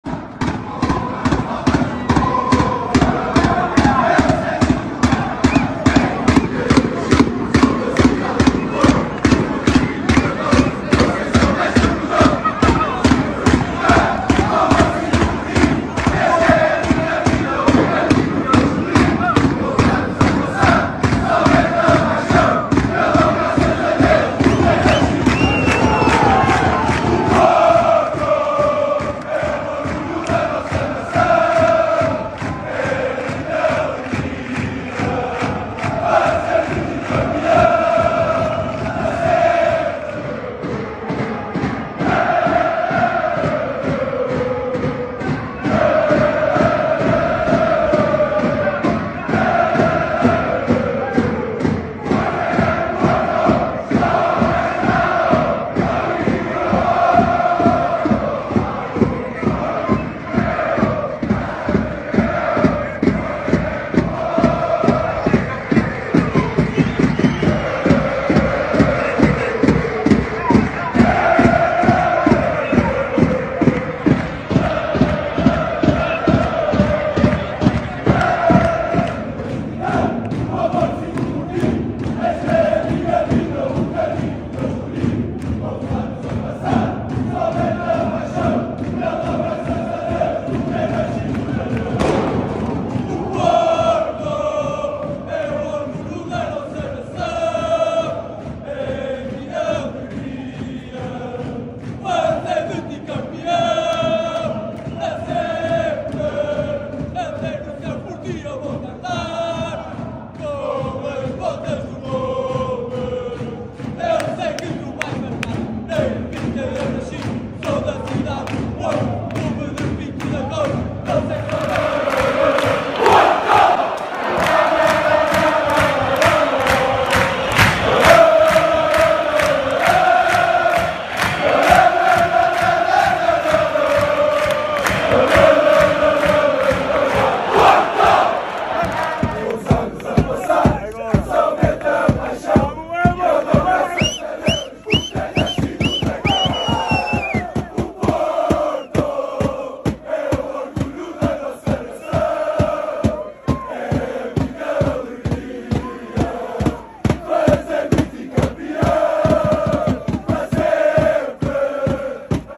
Benfica vs Fc Porto 2-1 | CORTEO PORTO FANS IN BENFICA | 07-04-2023
MUST SEE! Fc Porto fans with an incredible corteo in Benfica before de match against Benfica. The porto fans where loud and clear before and during the match! Awesome support!

DEVE VER! Torcedores do Fc Porto com cortejo incrível no Benfica antes da partida contra o Benfica. Os torcedores do Porto falaram alto e bom som antes e durante a partida! Suporte incrível!

¡DEBES VER! Aficionados del FC Porto con un increíble corteo en el Benfica antes del partido contra el Benfica. ¡Los aficionados del Porto hablaron alto y claro antes y durante el partido! ¡Impresionante apoyo!

SEHENSWERT! Fans des FC Porto mit einem unglaublichen Corteo in Benfica vor dem Spiel gegen Benfica. Die Porto-Fans waren vor und während des Spiels laut und deutlich! Tolle Unterstützung!